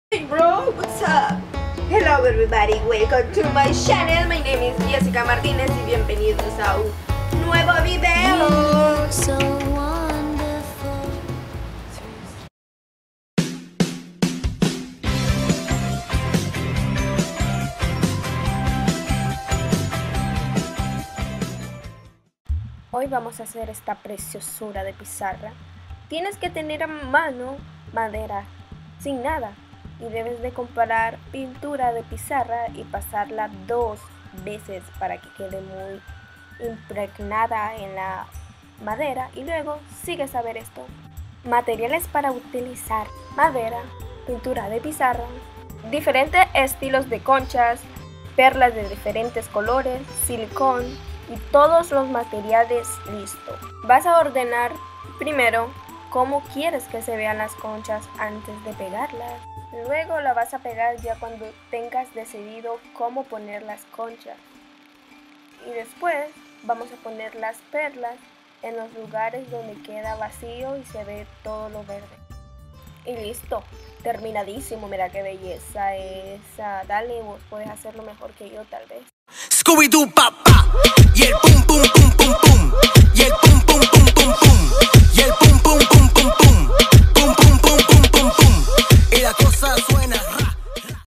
DECORA UNA PIZARRA CON CONCHAS / Jessica Martinez
hola belleza hoy traigo esta idea bellisima para decorar
como es una pizarra espero te guste bye.